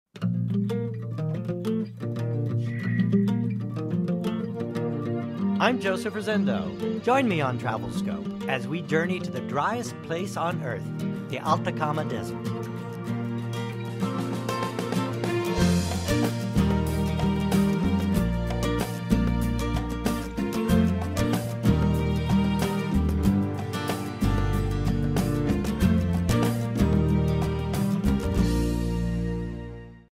Chile - The Driest Place on Earth
Joseph travels north to the driest place on the planet, Chiles Atacama Desert.  He finds that the desert, though it appears dry and barren, is alive with varied wildlife and vibrant cultures.  During his journey he encounters the friendly Atacameño Indians and witnesses spouting geysers, endless sand dunes and the areas breathtaking natural beauty.